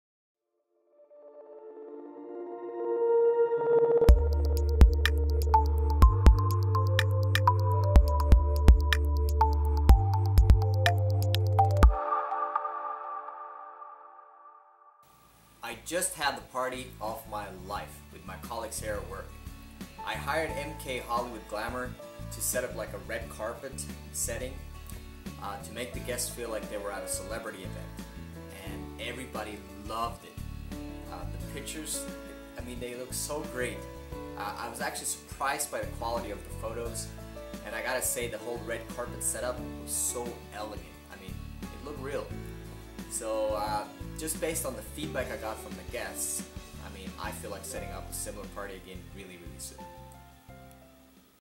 MK Hollywood Glamour Review - Mike J.
Here at MK Magnets nothing makes us happier than a happy customer. We go to the great lengths to make sure everyone of our customers is more than satisfied with our service. Check out this recent video testimonial from Mike Jensen who have used our MK Hollywood Glamour Red Carpet Service. Thanks Mike!